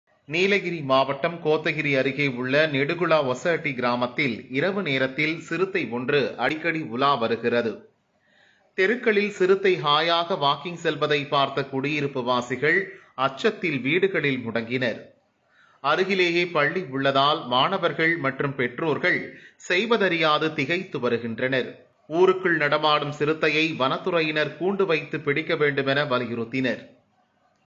தெருக்களில் சிறுத்தை உலா குடியிருப்பு வாசிகள் அச்சம்
நீலகிரி மாவட்டம் கோத்தகிரி அருகே உள்ள நெடுகுளா ஒசஹட்டி கிராமத்தில் இரவு  நேரத்தில் சிறுத்தை ஒன்று அடிக்கடி உலா வருகிறது.

தெருக்களில் சிறுத்தை ஹாயாக வாக்கிங் செல்வதை பார்த்த குடியிருப்பு வாசிகள் அச்சத்தில் வீடுகளில் முடங்கினர்.

அருகிலேயே பள்ளி உள்ளதால்  மாணவர்கள் மற்றும் பெற்றோர்கள் செய்வதறியாது திகைத்து வருகின்றனர்.

ஊருக்குள் நடமாடும் சிறுத்தையை வனவத்துறையினர் கூண்டு வைத்து பிடிக்க வேண்டும் என வலியுறுத்தினர்.